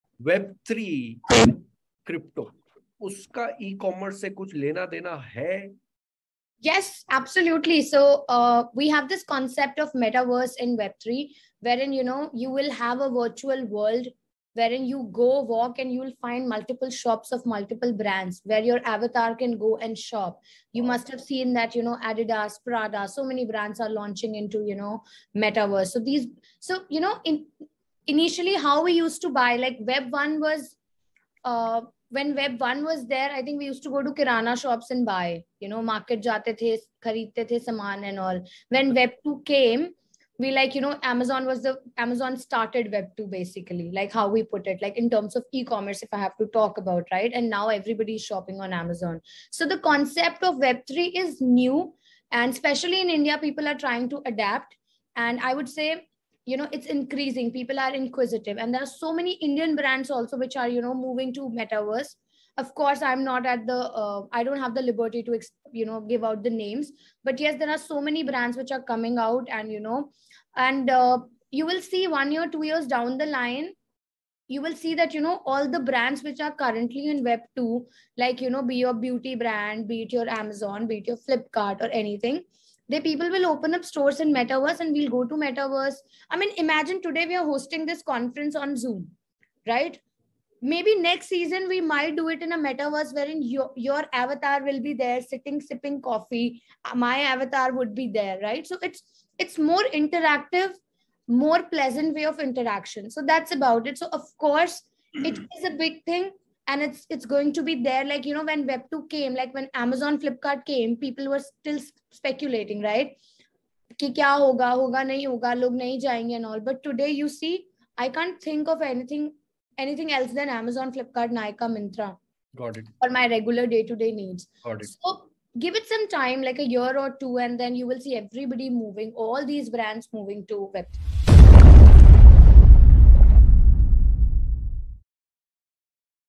Does Ecommerce have a future in WEB3?
While discussing eCommerce on the 1K Founders show, we discussed with Annu who is part of @polygonstudios1186 if there is a future for Ecommerce in Web3. This is what she said.

We present the 1K Founders show, in collaboration with @Shiprocket